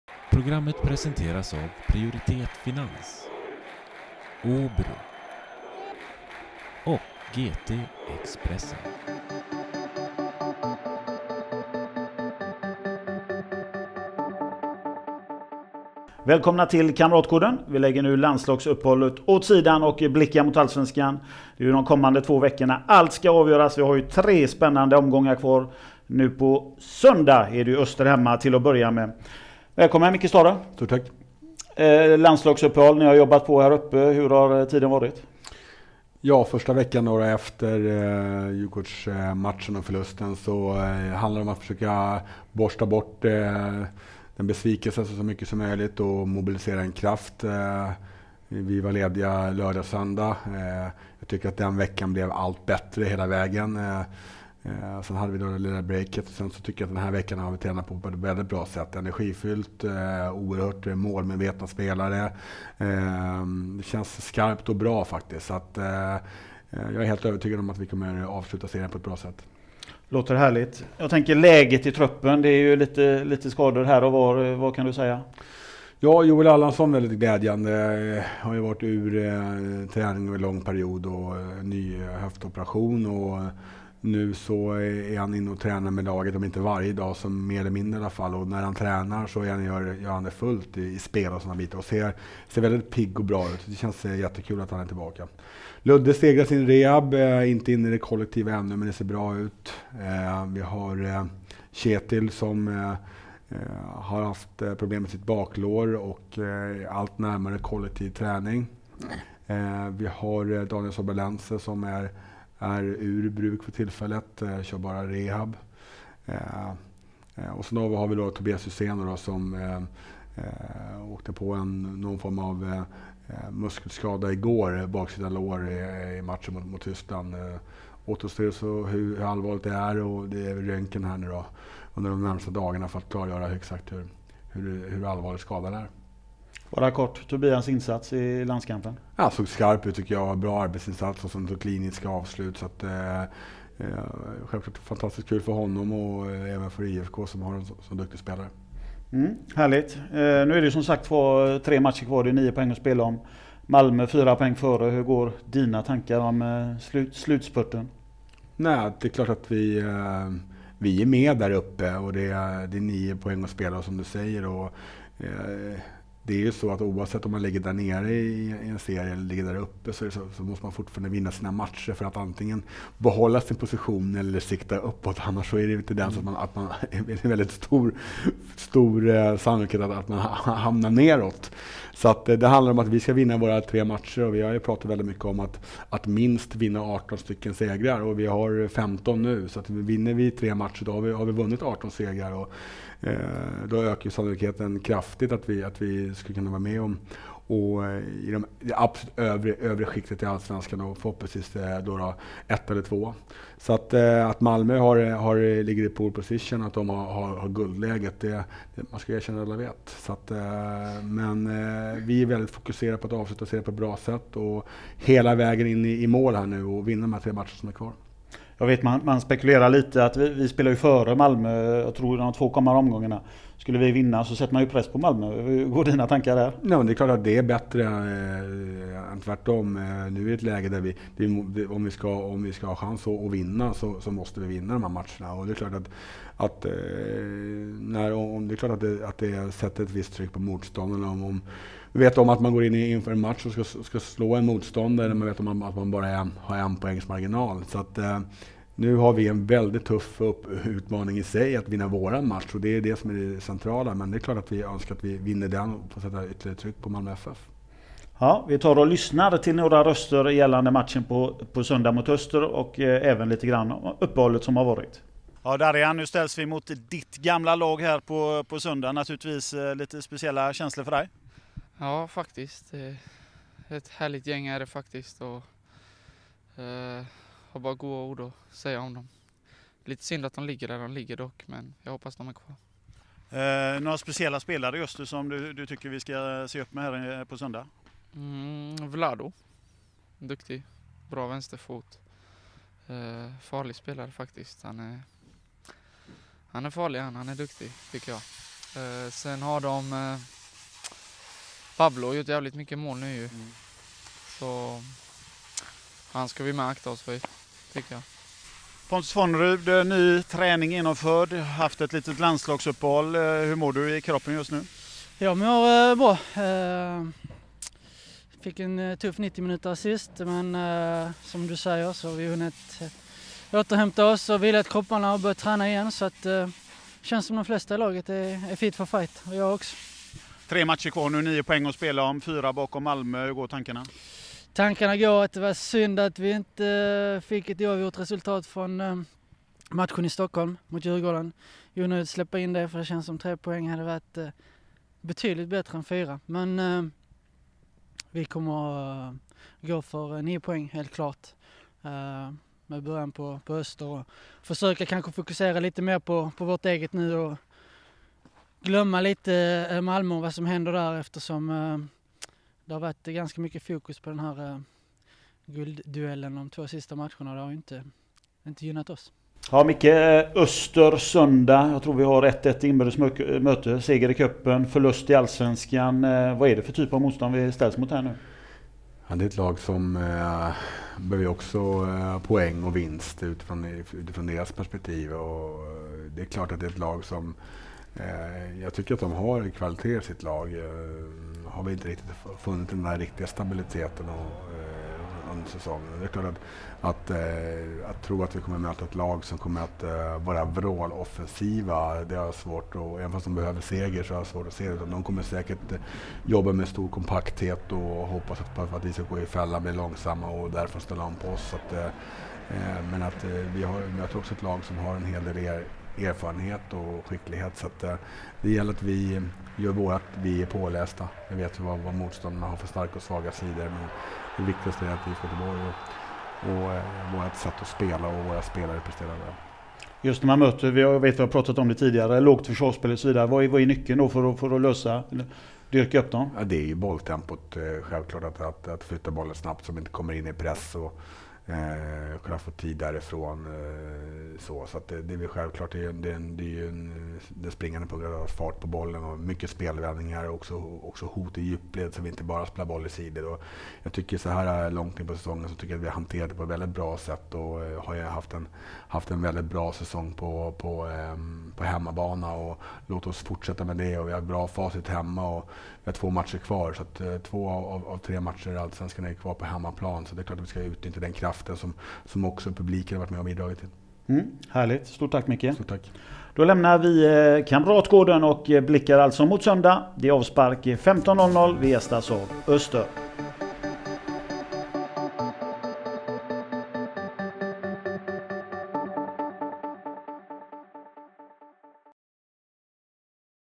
Uppladdning inför matchen mot Öster
Mikael Vikla har besök i studion av tränare Mikael Stahre, tillsammans pratar dom upp matchen mot Öster som spelas på Gamla Ullevi på söndag.